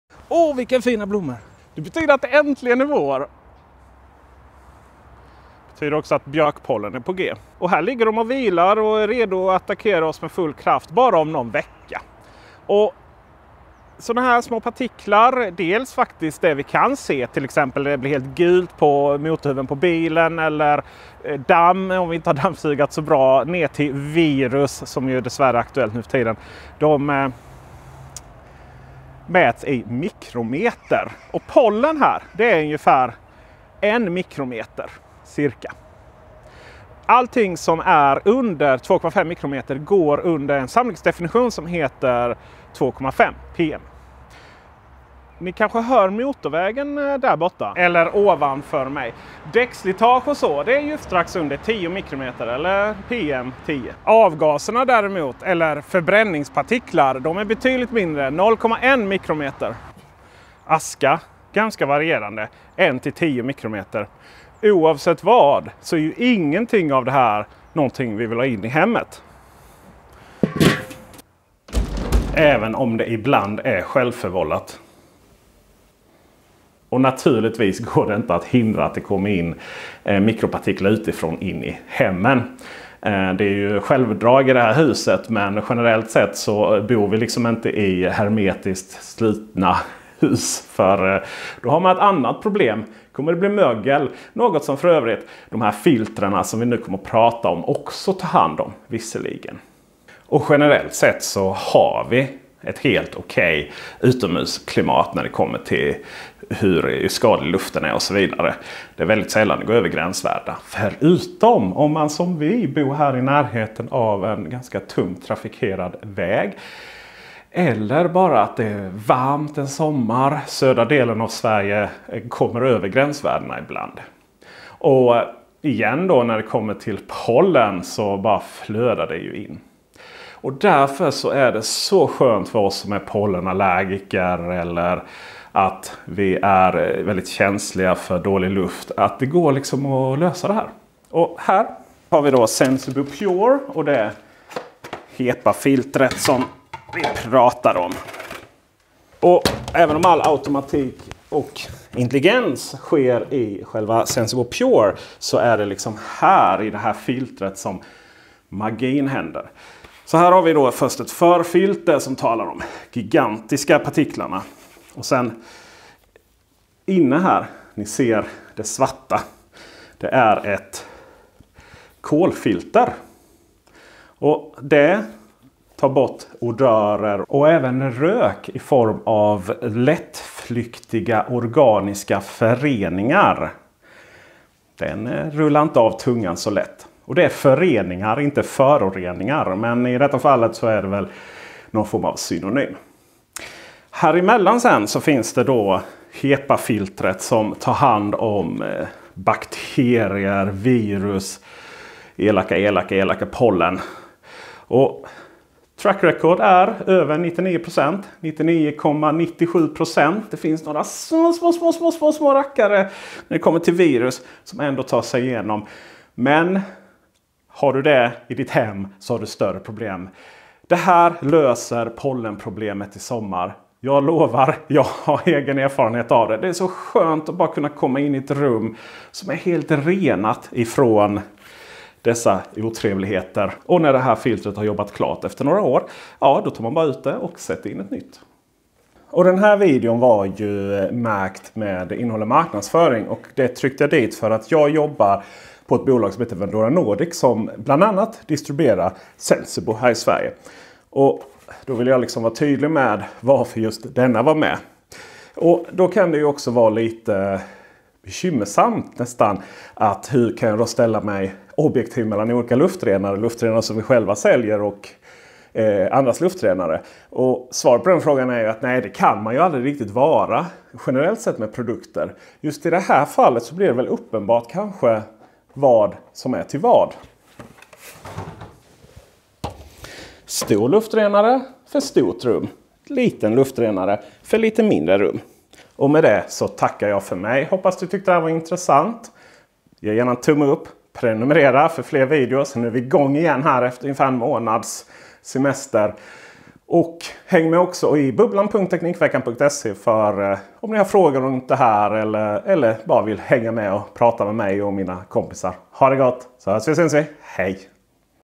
Så fungerar luftrenare
Snart är det sol, sommar och en helskotta massa pollen i luften! Luftrenare gör jobbet och så här fungerar de.